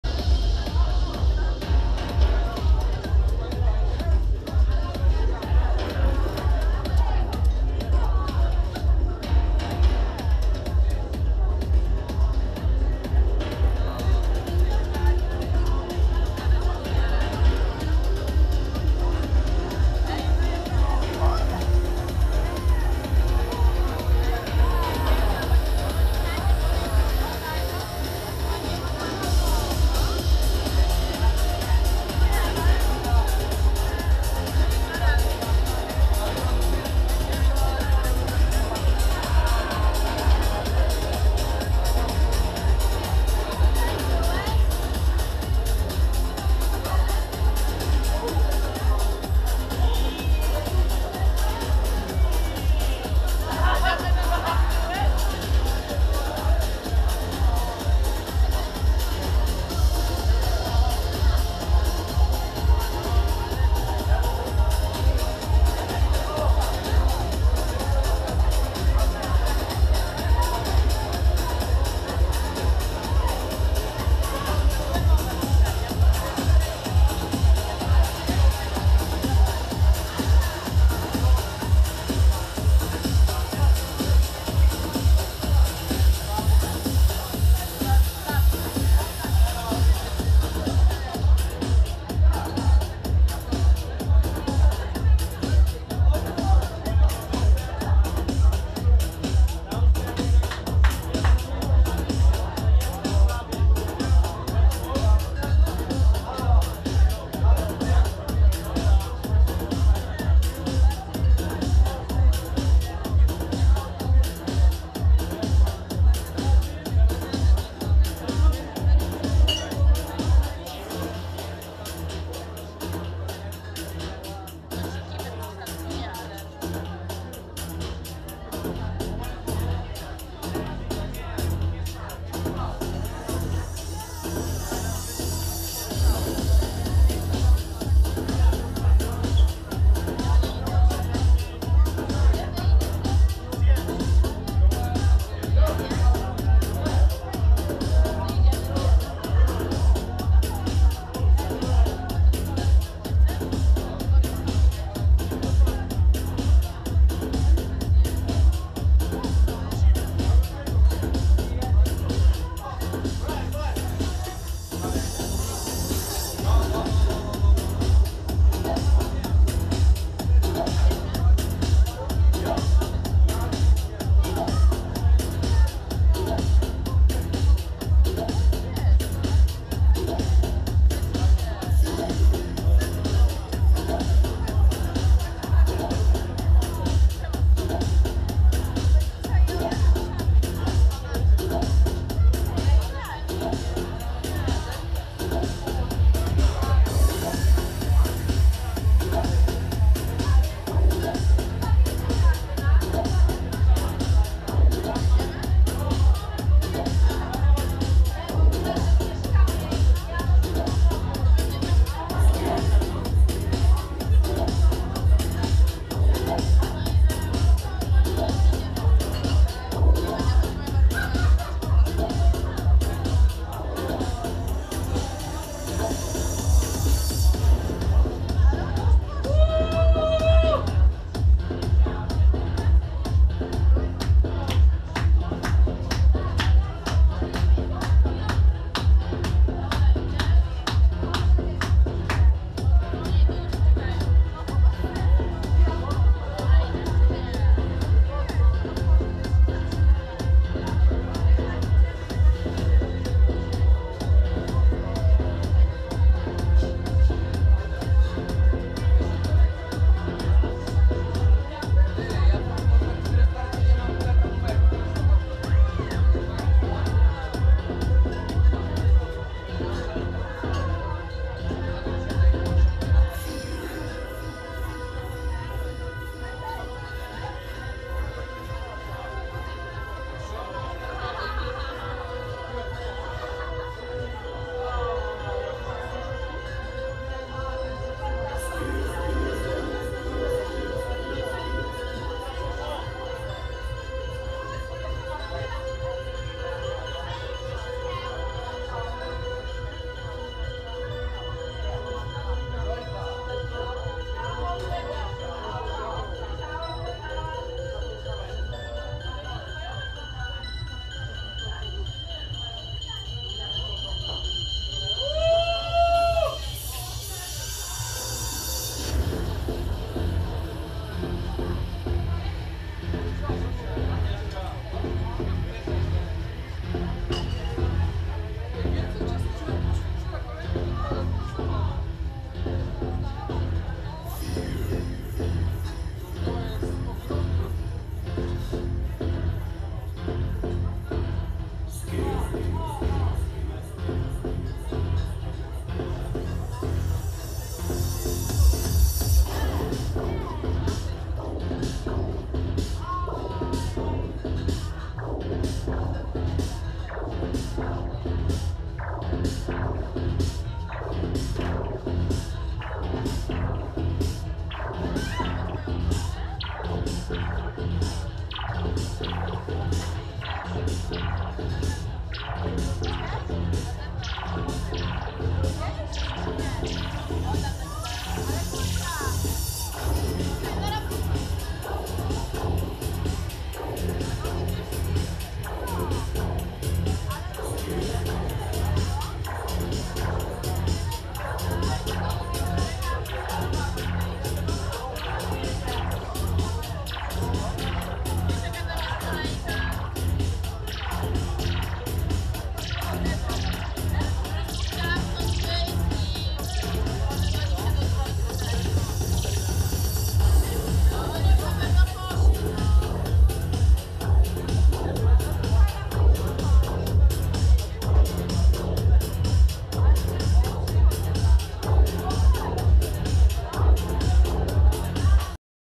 RAPID FIRE - Powrót do Źródła vol. 12 - Poznań - Goa Trance Party @ Projekt Lab
ॐ Scena Groove - bar (Tribal, Deep & Dark Progressive House):
01:30 - 03:00 RAPID FIRE